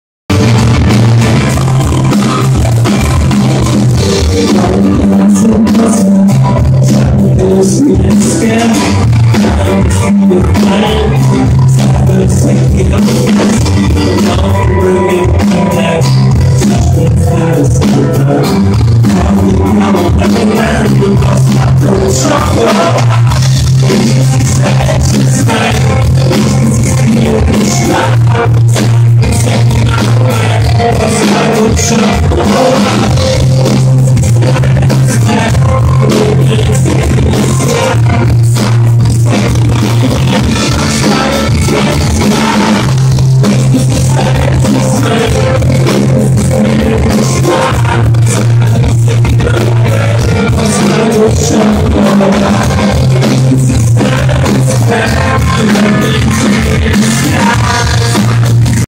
LG G2 sample video, low light and loud
The LG G2's camera captures some pretty great video in normal conditions, but what if we push it out of its comfort zone, into a closed-up club space, with low light, a lot of reflections and eardrum-shattering sound?